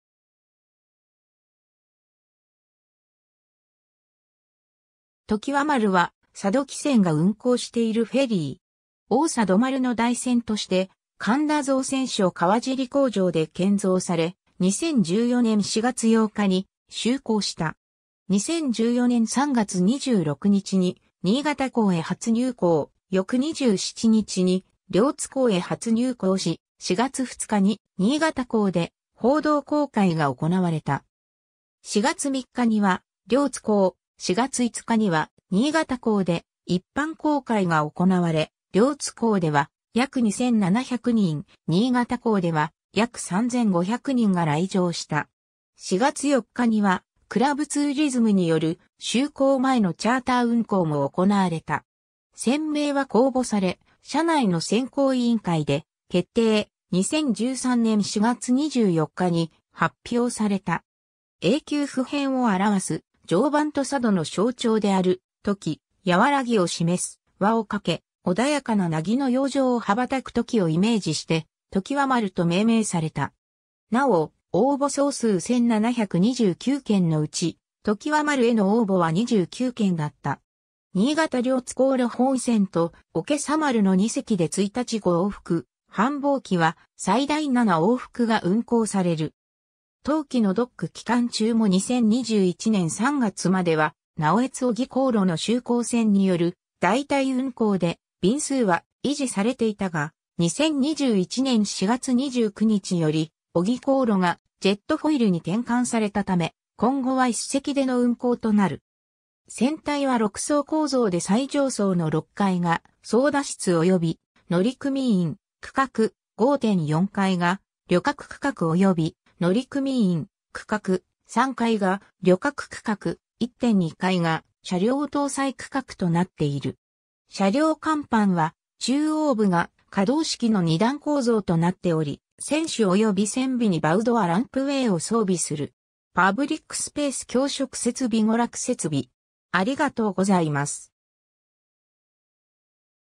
ときわ丸
ときわ丸（ときわまる）は、佐渡汽船が運航しているフェリー。
 おおさど丸 (2代)の代船として神田造船所川尻工場で建造され、2014年4月8日に就航した。
 2014年3月26日に新潟港へ初入港、翌27日に両津港へ初入港し、4月2日に新潟港で報道公開が行われた。
4月3日には両津港、4月5日には新潟港で一般公開が行われ、両津港では約2700人、新潟港では約3500人が来場した。
4月4日には、クラブツーリズムによる就航前のチャーター運航も行われた。
 船名は公募され、社内の選考委員会で決定、2013年4月24日に発表された。
永久不変を表す「常磐」と佐渡の象徴である「朱鷺」、和らぎを示す「和」をかけ、穏やかな凪の洋上を羽ばたくトキをイメージして、「ときわ丸」と命名された。
なお、応募総数1,729件のうち、「ときわ丸」への応募は29件だった。
 新潟両津航路 本船とおけさ丸 (3代)の2隻で1日5往復、繁忙期は最大7往復が運航される。
冬期のドック期間中も2021年3月までは直江津-小木航路の就航船による代替運航で便数は維持されていたが、2021年4月29日より小木航路がジェットフォイルに転換された為、今後は1隻での運航となる。
 船体は6層構造で最上層の6階が操舵室および乗組員区画、5・4階が旅客区画および乗組員区画、3階が旅客区画、1・2階が車両搭載区画となっている。
 車両甲板は中央部が可動式の2段構造となっており、船首および船尾にバウドア・ランプウェイを装備する。
 パブリックスペース 供食設備 娯楽設備